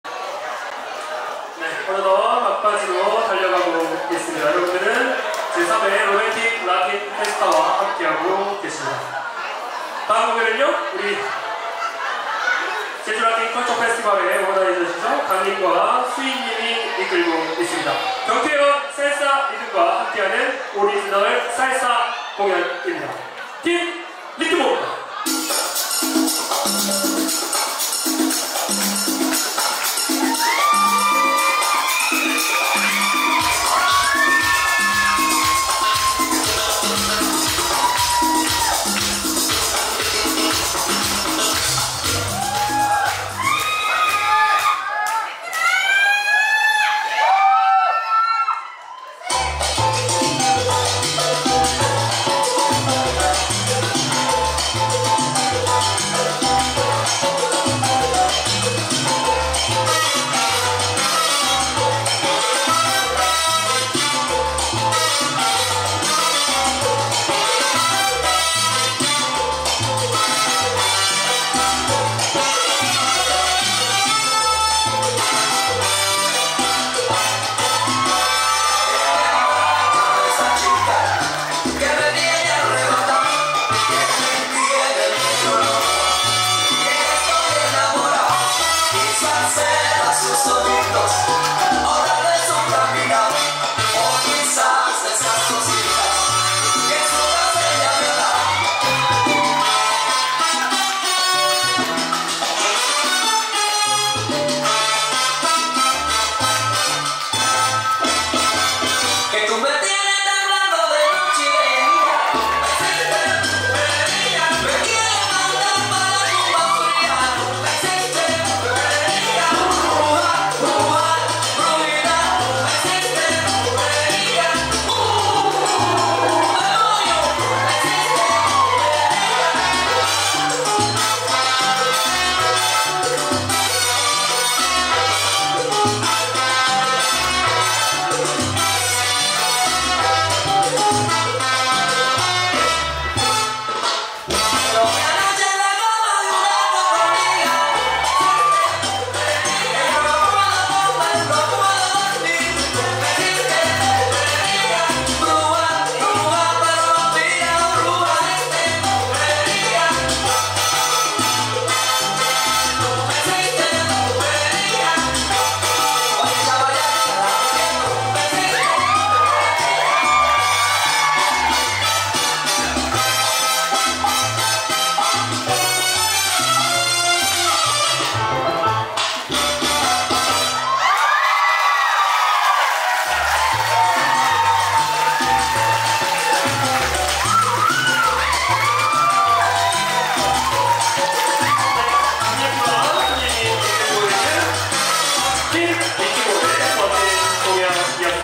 리트모 by 각&수임 살사 공연 @ 3rd Yeosu Romantic Latin Festa 26-28 April 2024
2024년 4월 27일(토)
DJ 리토 /여린 / 헤이즐

in 여수 라마다호텔

다른곳에 업로드시 촬영자 표시를  해 주시기 바랍니다.

카카오톡 : gandi11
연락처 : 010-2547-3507